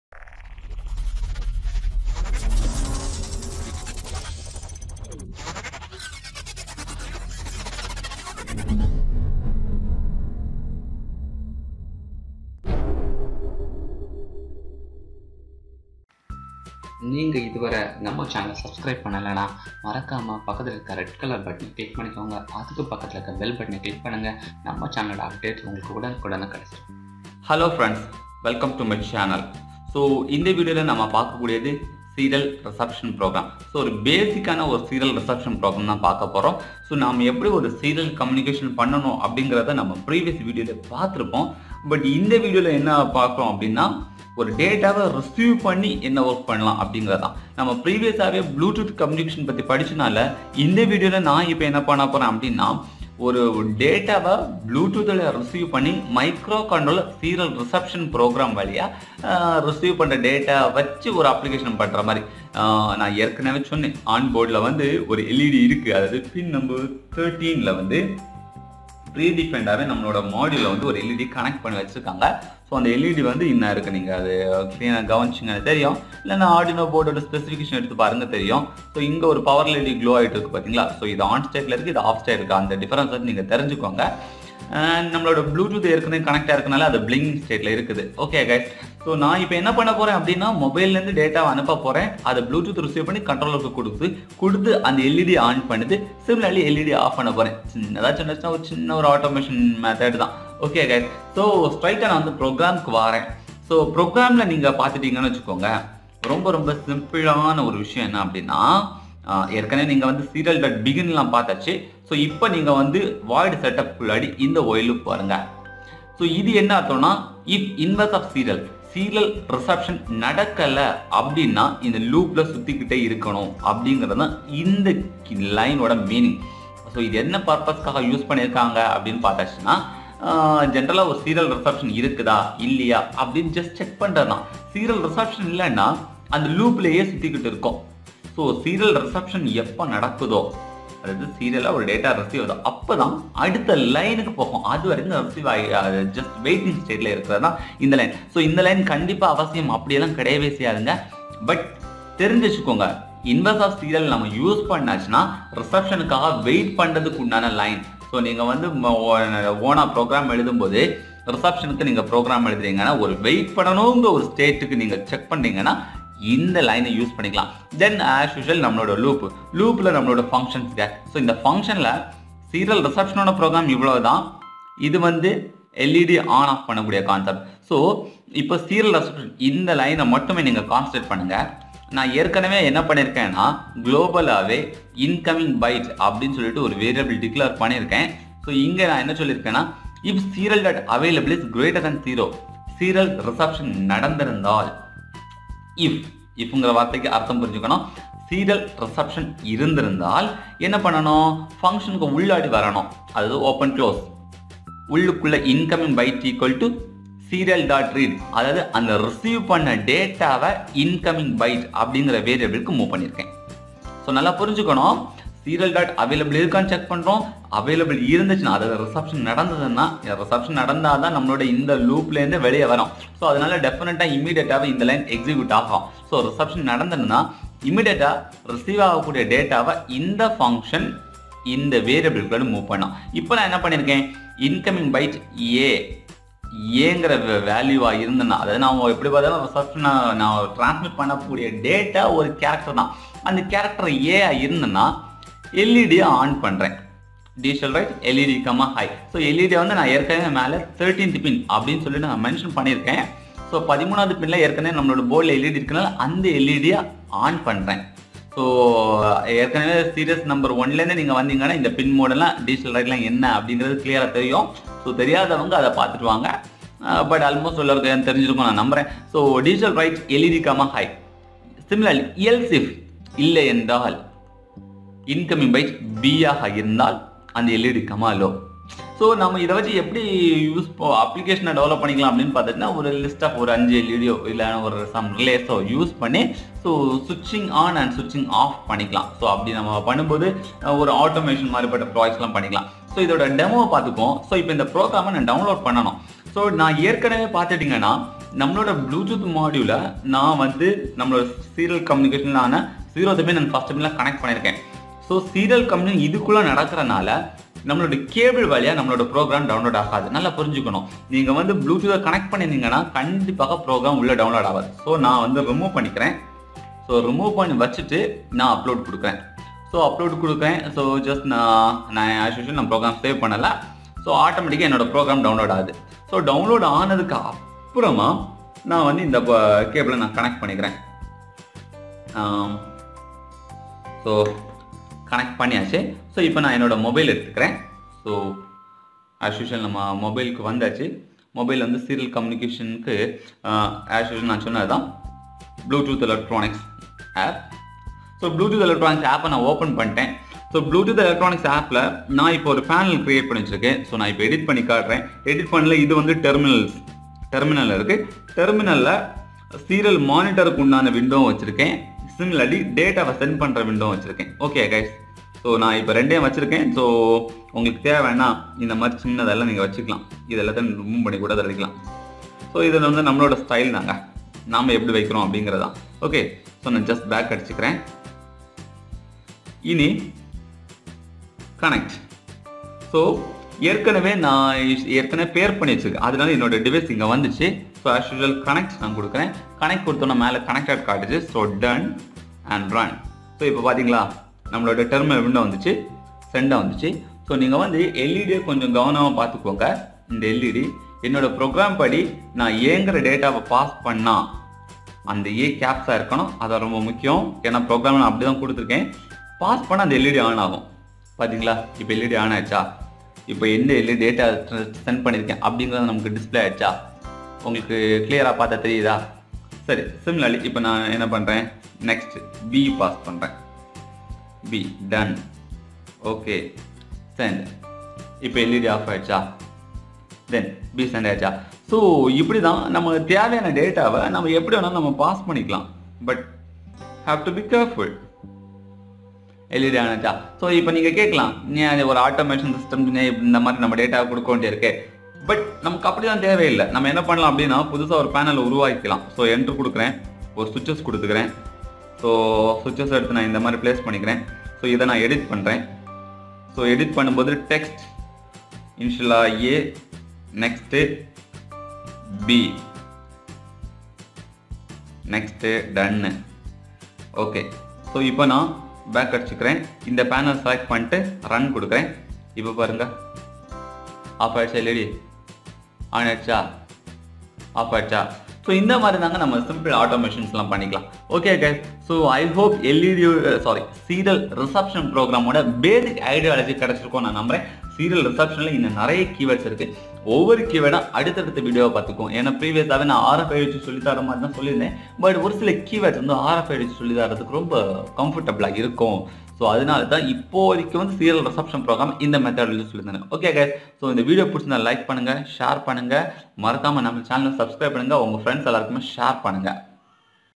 Automate an LED with android | Serial reception programming | arduino tamil tutorial
Lets learn the serial reception programming.
code available in the below description